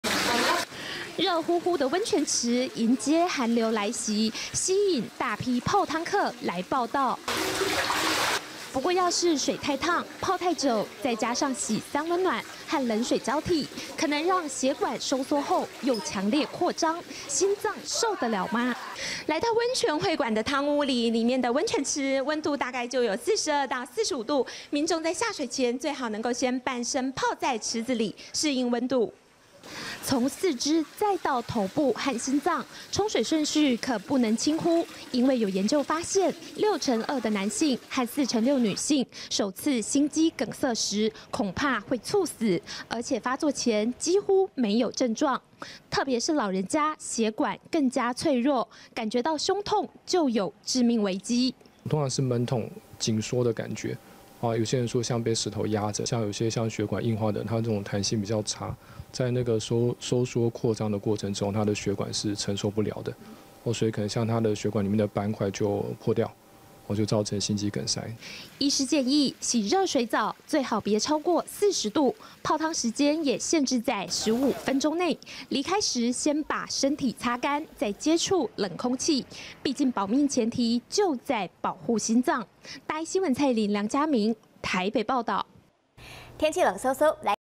泡湯SOP 護心也保命
入冬第一個寒流即將報到，許多人可能會洗熱水澡，或是去泡湯來暖身，但老年人或有三高疾病的患者，要特別注意血管彈性較差，醫師提醒，胸部如果忽然有悶痛感，就可能是心肌梗塞的警訊，平時沖熱水，最好別超過40度，避免溫度變化太大，心臟難以負荷。